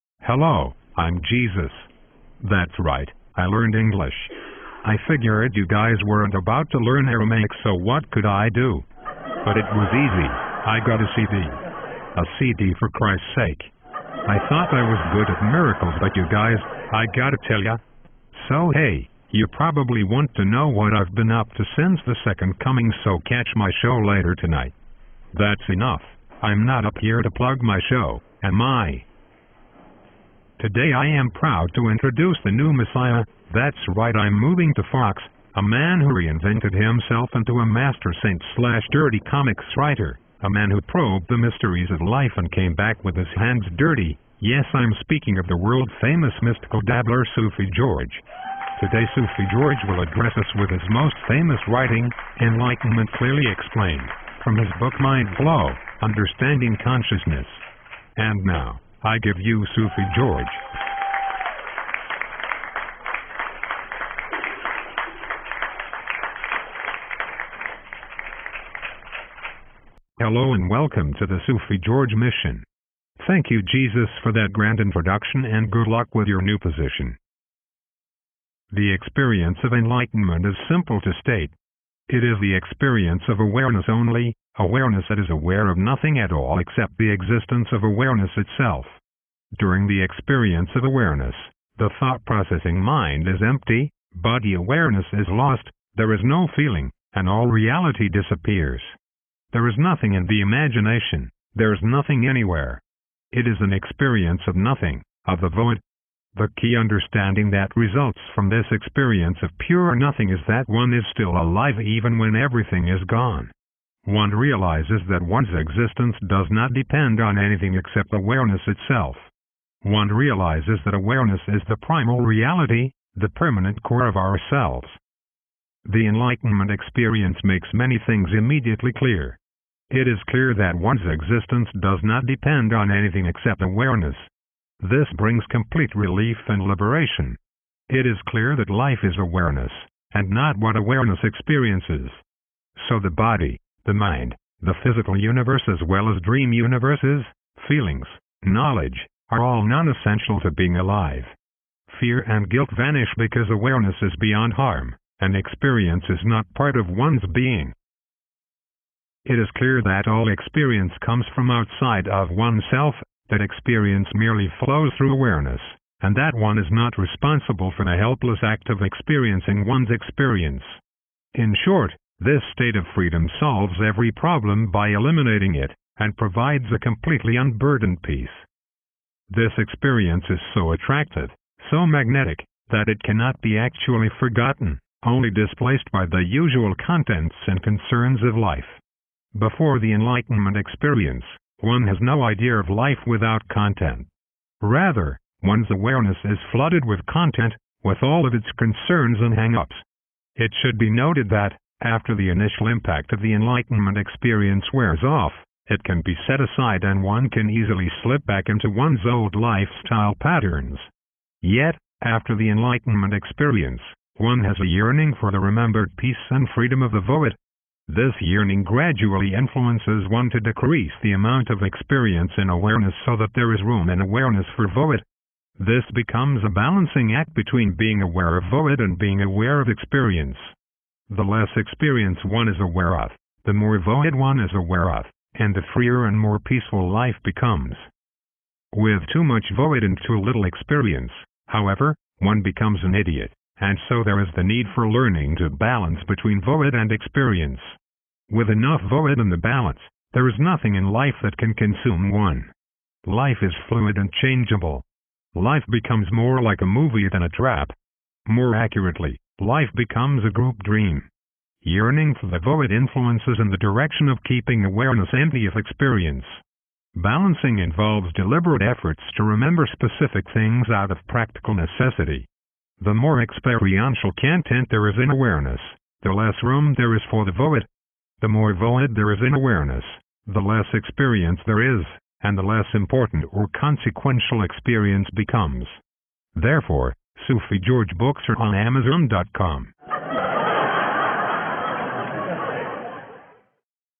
SGM Sufi George's famous Enlightenment Clearly Explained
Sufi George explains enlightment in his most famous article.